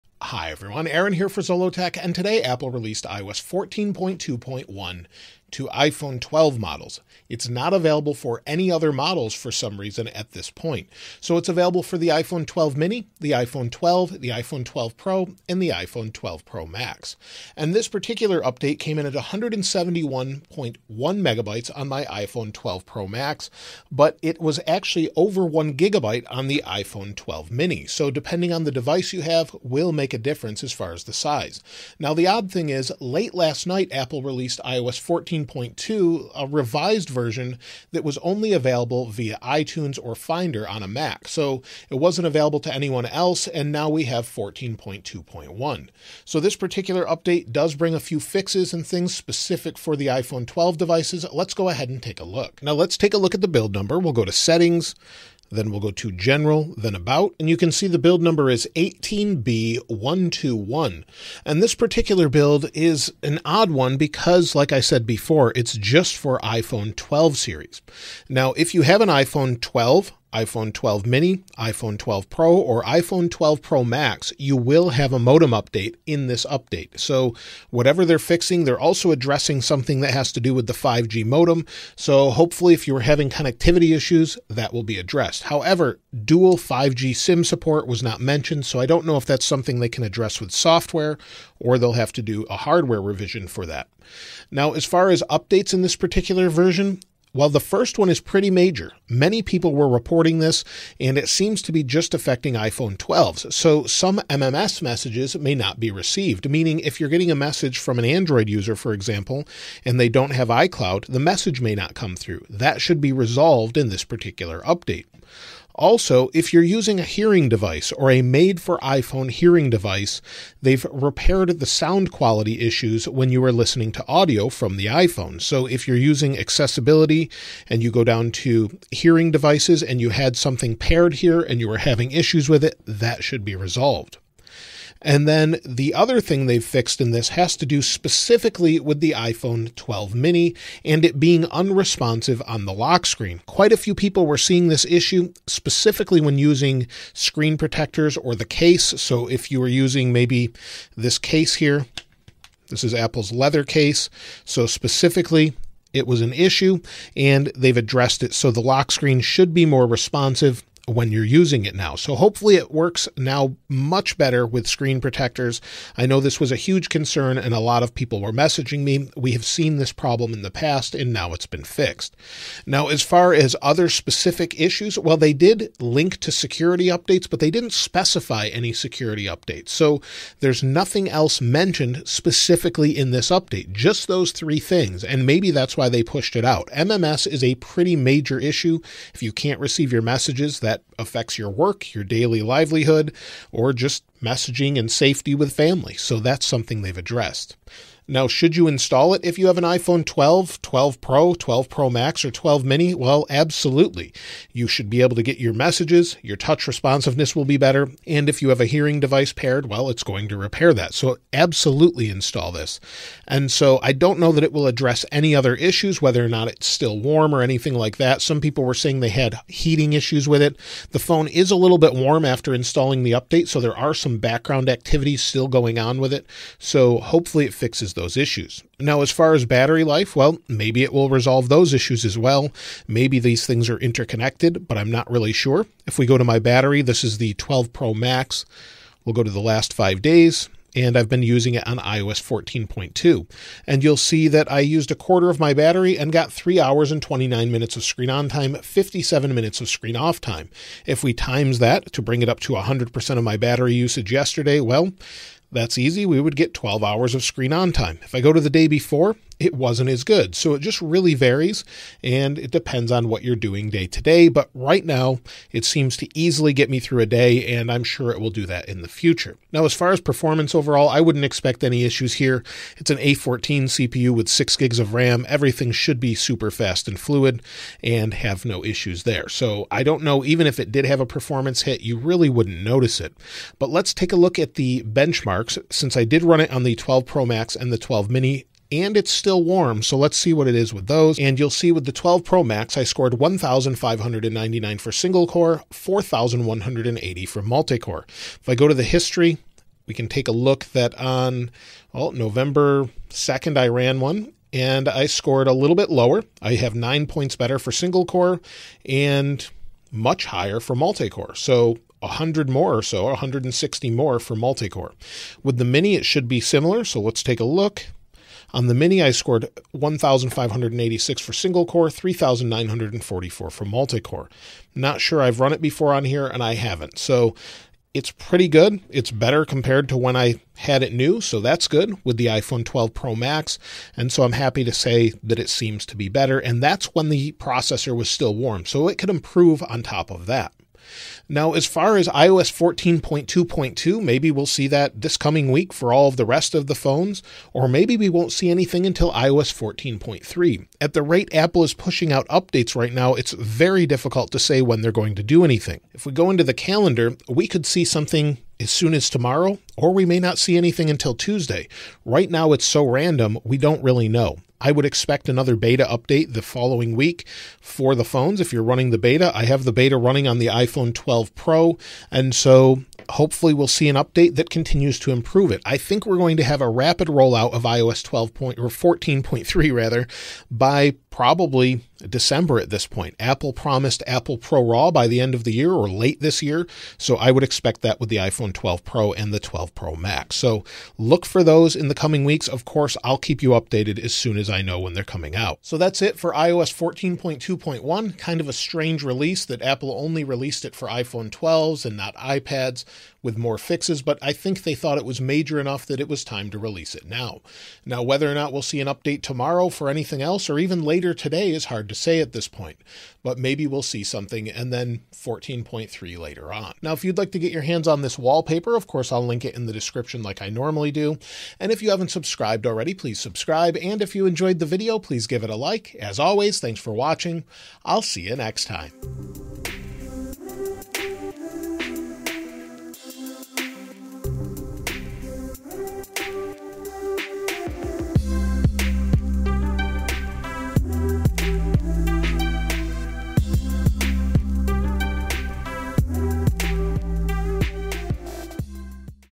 iOS 14.2.1 is Out! - What's New?
iOS 14.2.1 released for iPhone 12 series devices and brings with it a number of fixes for iPhone 12 mini, iPhone 12, iPhone 12 Pro and iPhone 12 Pro Max. Also, late last night Apple released iOS 14.2 as a revised version which can only be install using iTunes or Finder on a Mac. In this video I go over all the features, updates and changes in iOS 14.2.1 using, iPhone 12 Pro Max.

***Time Codes***
00:00 - Introduction
00:05 - Supported devices
00:17 - Size
00:31 - iOS 14.2 revised version
00:52 - Build number
01:14 - Modem Update
01:26 - Dual 5G sim cards
01:34 - New Features and Updates
02:52 - Security updates
03:17 - Should You Install iOS 14.2.1?
03:33 - Heat
03:48 - Battery
04:29 - Performance
04:43 - Benchmarks
05:40 - iOS 14.2.2 and iOS 14.3
06:35 - Conclusion
06:56 - Wallpaper
07:09 - Outro

iPhone 12 and iPhone 12 Pro Cases with MagSafe - Unboxing and Everything You Wanted To Know